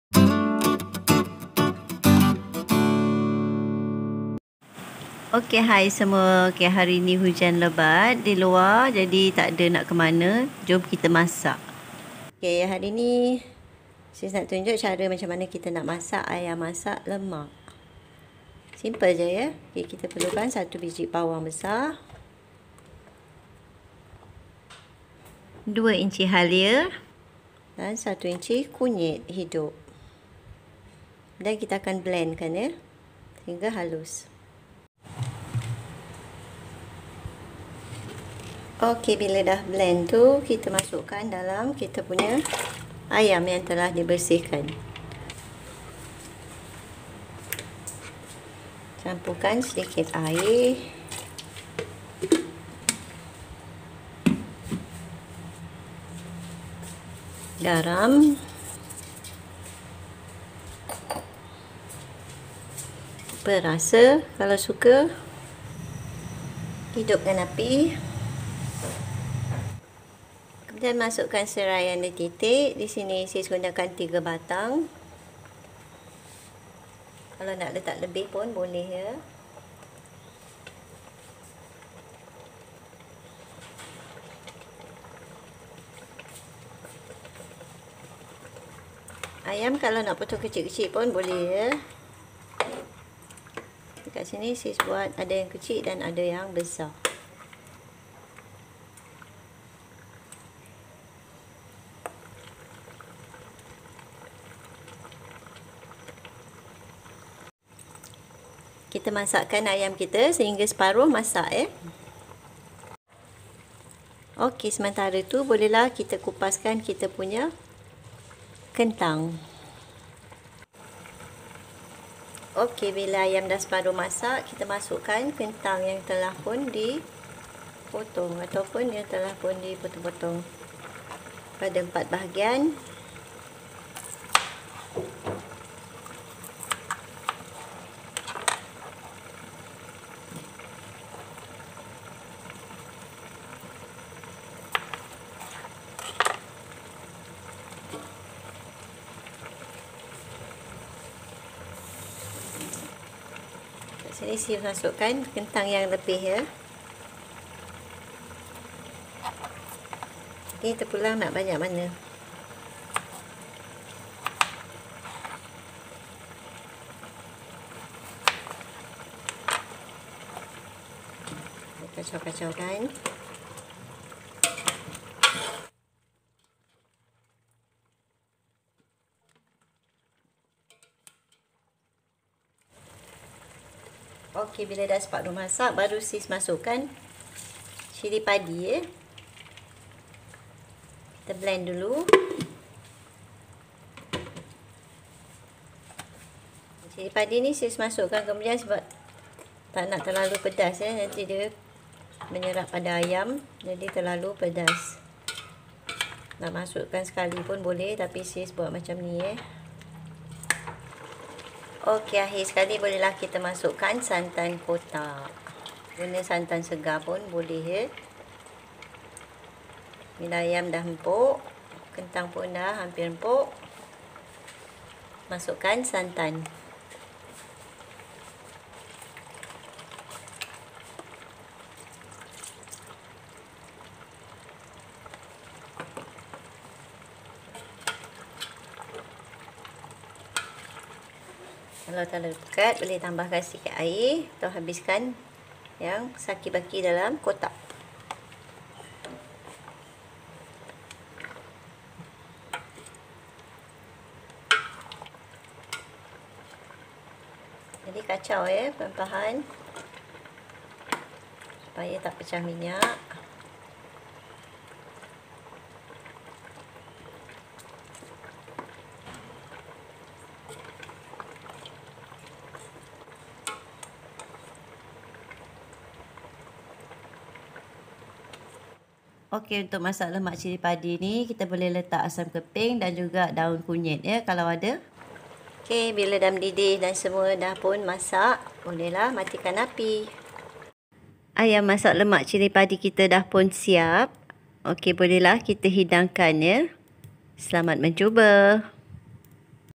Ayam Masak Lemak Cili Padi | Untuk pemula yang baru belajar masak
Hai semua, hari ini sis kongsikan cara masak Ayam masak Lemak Cili Padi untuk pemula yang baru belajar memasak.
Bahan - bahan :
1 ekor ayam
1 labu bawang
2 inci halia
1 inci kunyit hidup
1 kotak santan ( nak guna santan segar pun boleh)
3 batang serai
Cili padi ( ikut kepedasan yang diinginkan)
Optional : Asam keping dan daun kunyit jika suka.
Garam
Perasa.
Nota* nak kisar dan masukkan cili padi sekali pun boleh masa mula - mula masak, sis tak boleh makan terlalu pedas, jadi masukkan cili padi setelah ayam separuh masak.